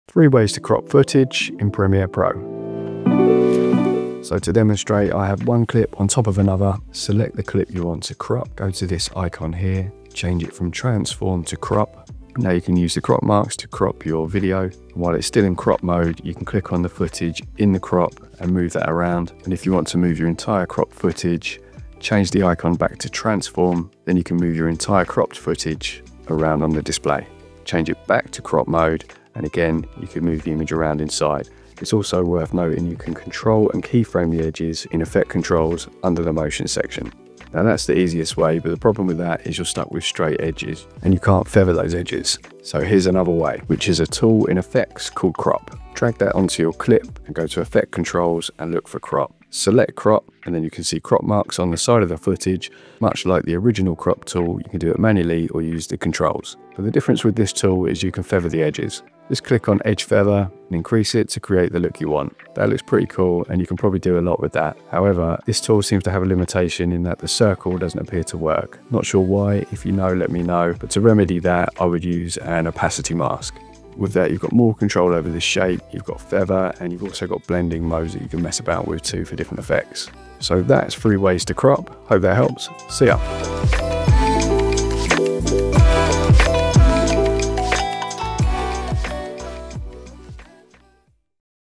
How To CROP VIDEO in Premiere Pro - 3 different ways.
Quick and simple 'How To' tutorial showing the 3 best methods to crop footage in Premiere Pro

If you found this video useful, feel free to grab me a coffee!

Peace!